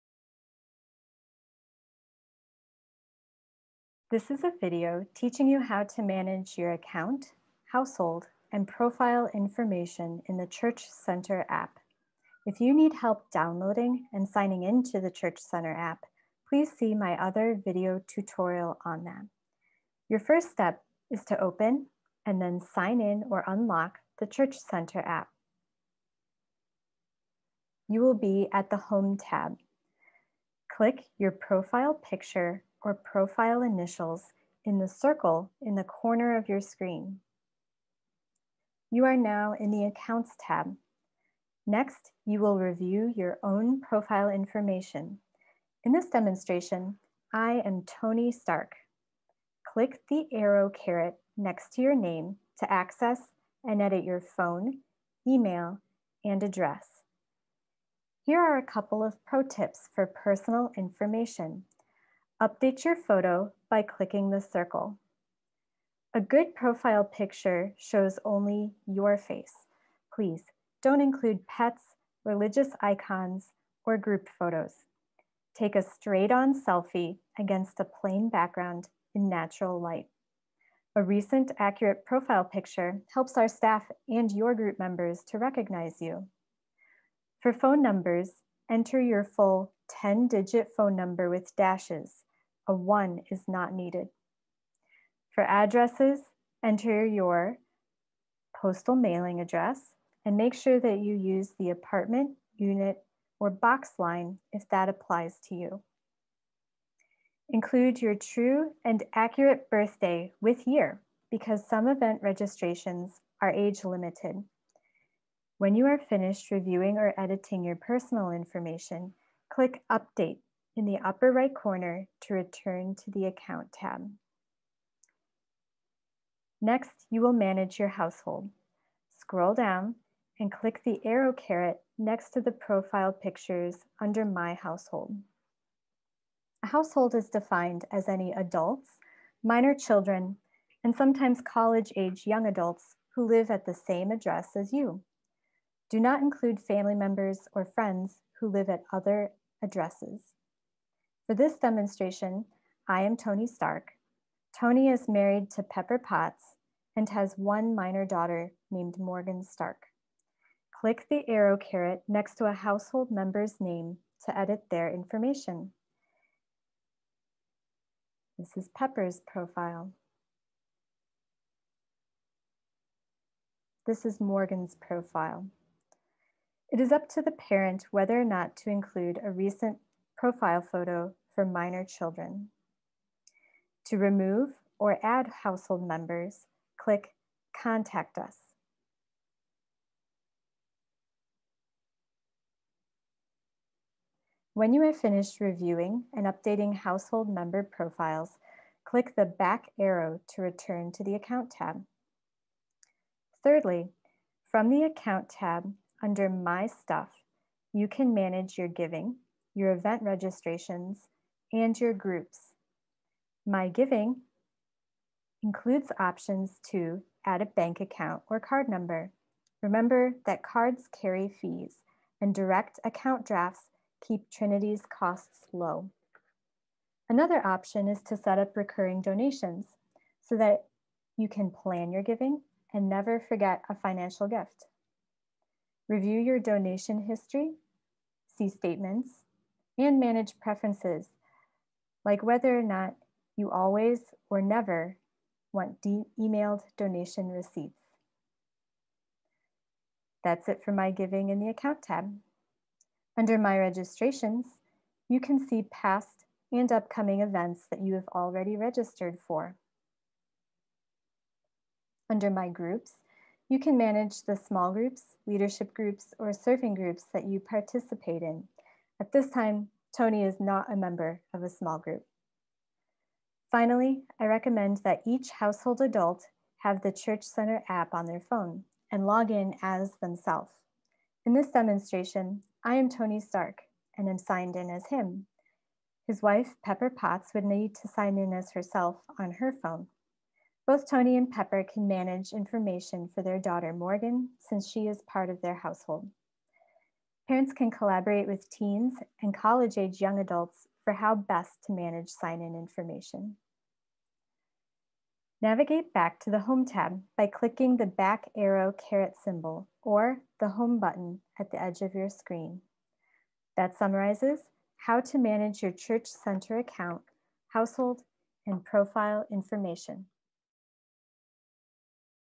Manage Your Account and Profile
How to manage your account and profile in the Church Center app.

You will need access to your phone's app store (Apple App Store or Google Play Store). The app is free, but you may need your App Store password or phone pin to complete download.